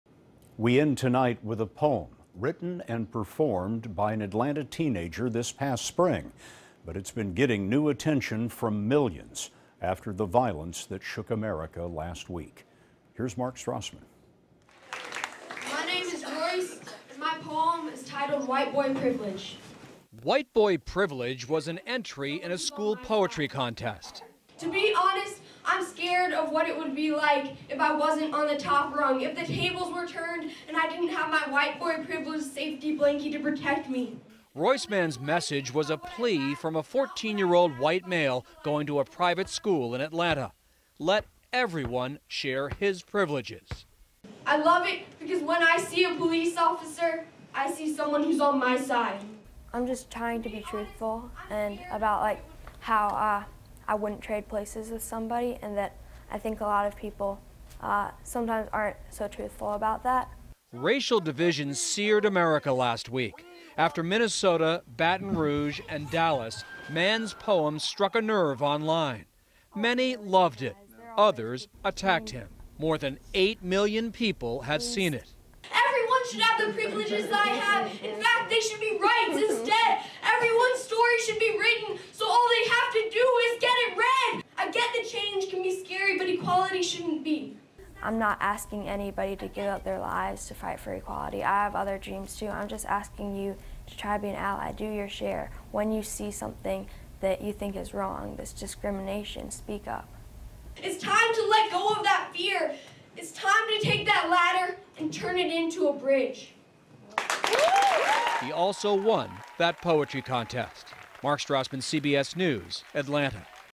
Eighth grader's white privilege poem goes viral
A poem written and performed by an Atlanta teenager this past spring has been getting new attention from millions since the violence that shook America last week. Mark Strassmann spoke with the 14-year-old about what inspired him to write it.